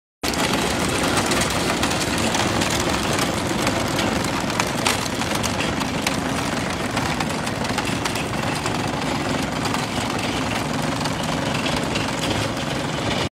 Accident grav pe podul peste cale ferată din Bucovăț, drumul R1 Ungheni - Chișinău. Part 2
Trei automobile s-au ciocnit violent pe podul peste calea ferată Chișinău - Ungheni din or. Bucovăț, parte a traseului național R1 Ungheni - Chișinău. În urma impactului, doi dintre șoferi au fost răniți, fiind transportați de urgență la spital.

sursa: Dionis Ternovschi